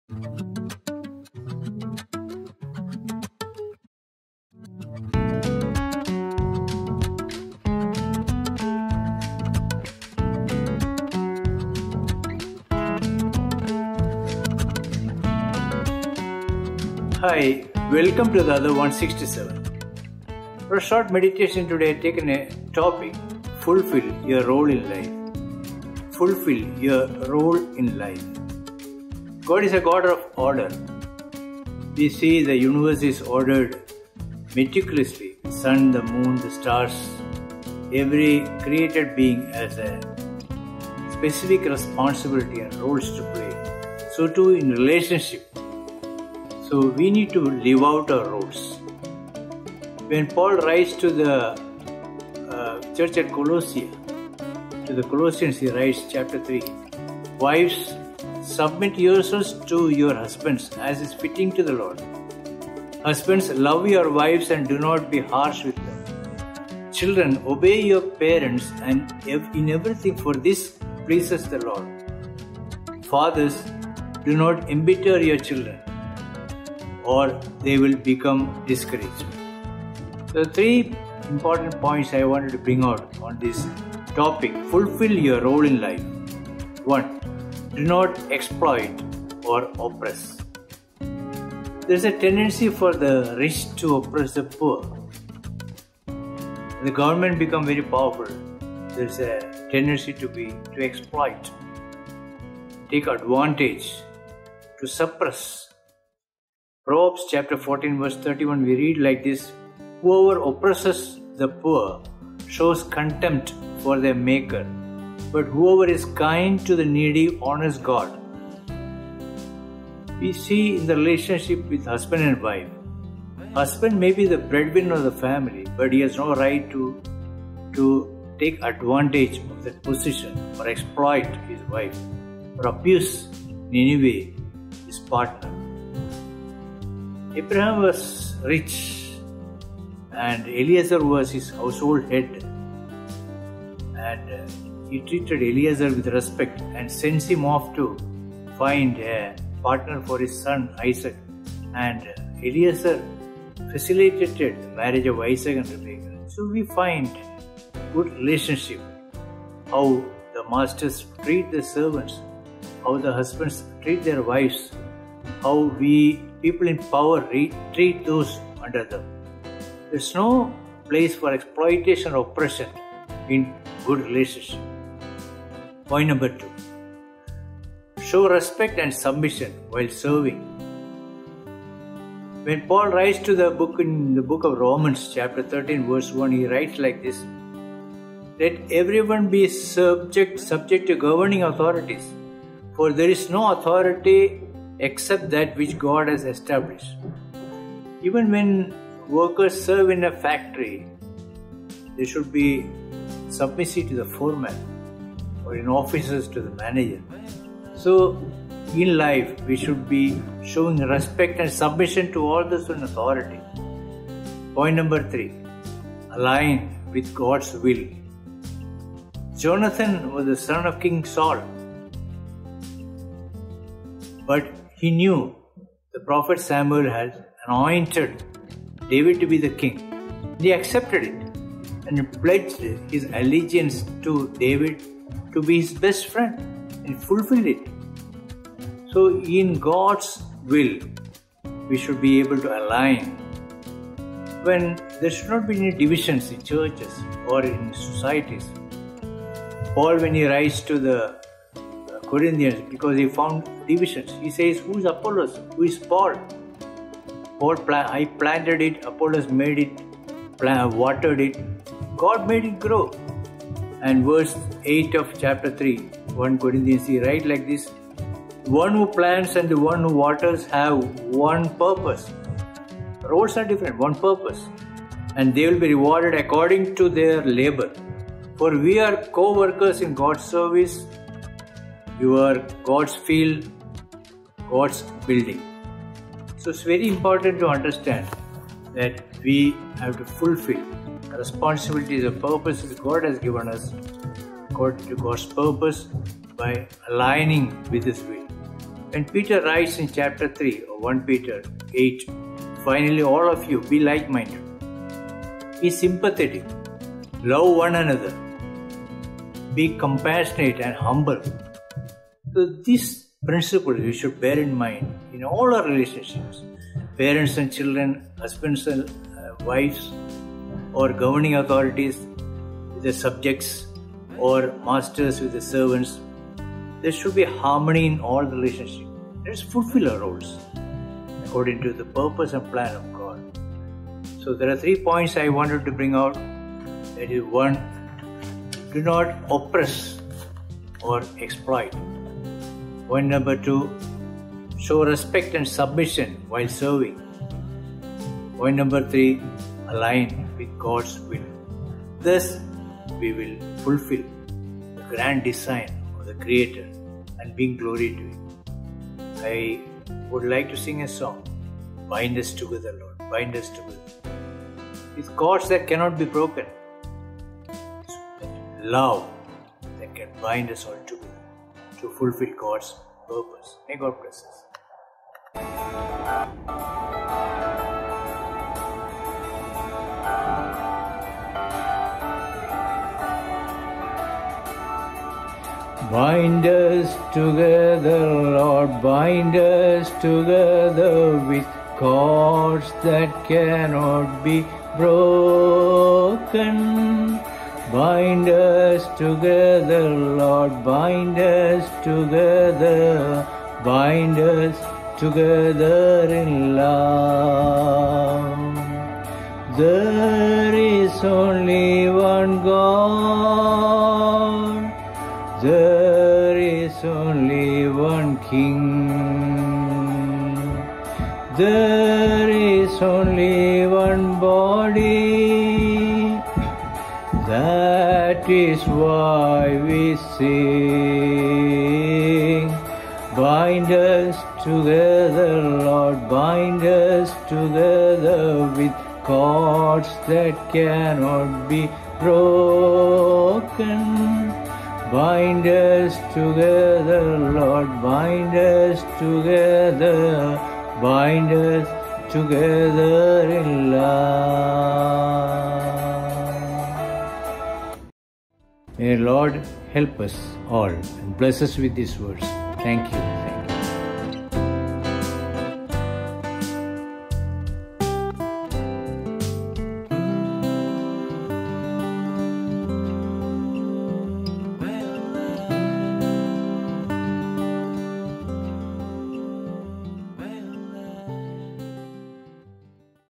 FULFILL YOUR ROLE IN LIFE | RAJAN MATHEW | THE OTHER 167
DAY 1372
September 29, 2024
Sunday

Title :-  Fulfill Your Role in Life

Bible Verse
Colossians 3:18-21

Bible Reading:
Colossians 3:18-25

 👉 While fulfilling the role in our life, we should not exploit or oppress

👉 Show respect and submission while serving

👉 Align with God’s will

Let's tune in to Rajan Mathew – Gideons and Haggai Ministries to learn more about Fulfill Your Role in Life.

Check us out on Twitter :-

Godspeed!!

Credits:-
Video Editor:-Caleb J & Thomas Mathew

Wondershare - Edits, Audio, Clips